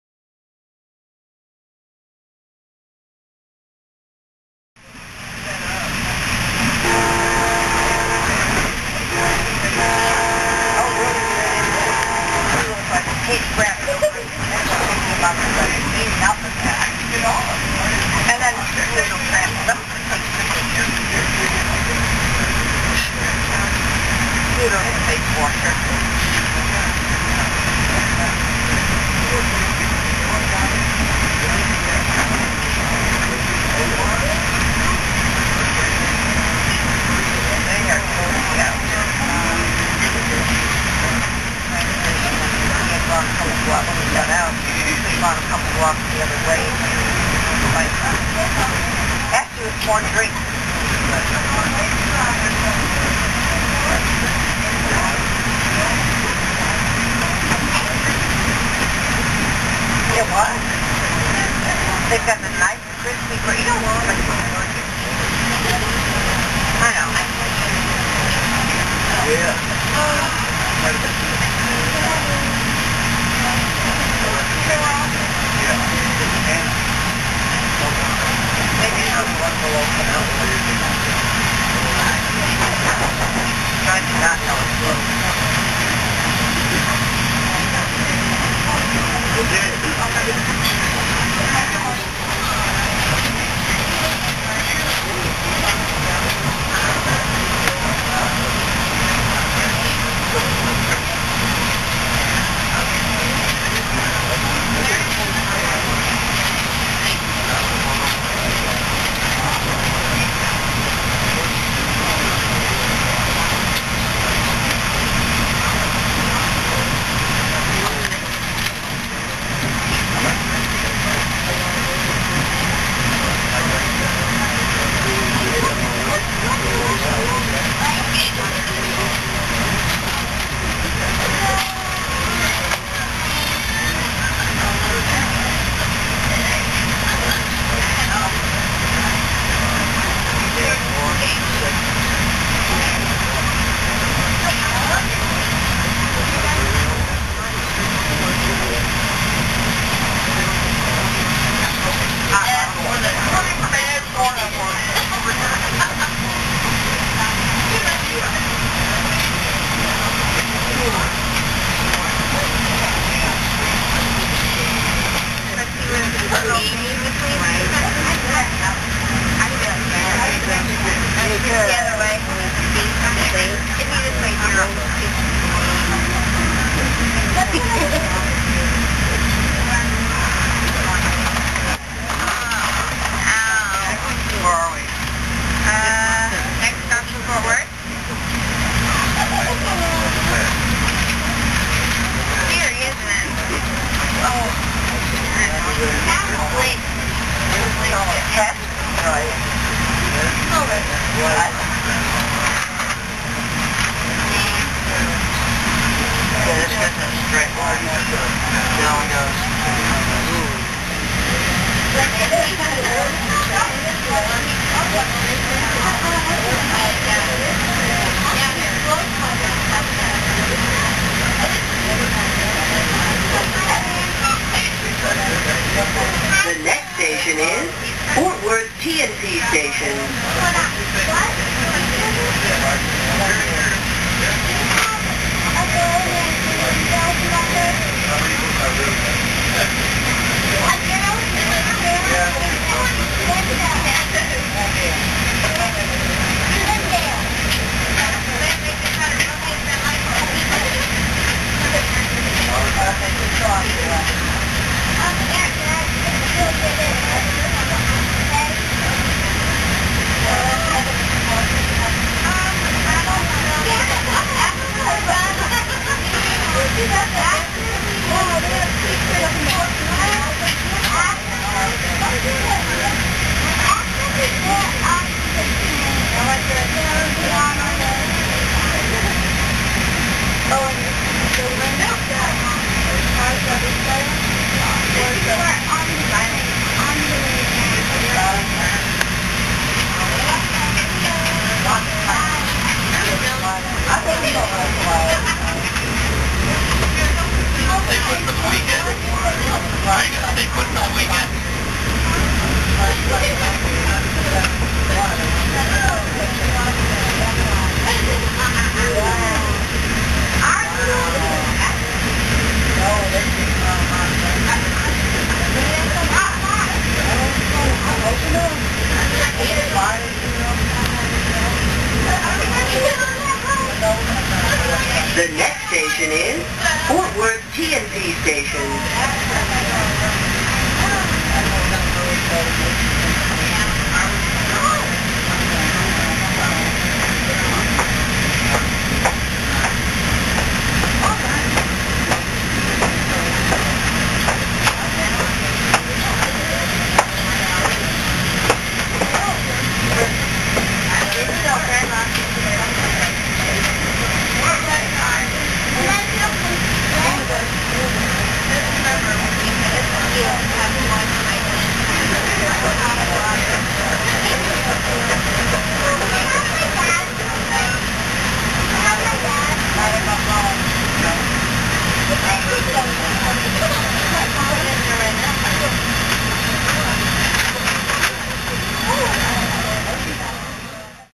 Riding the Trinity River Express - 06/30/06
This is a 6:20 minute compilation of 9 clips that I took from the Centerporte/DFW International Airport station to the Fort Worth T&P station on the TRE.  The view was looking out the left side of a Bombardier double-deck coach.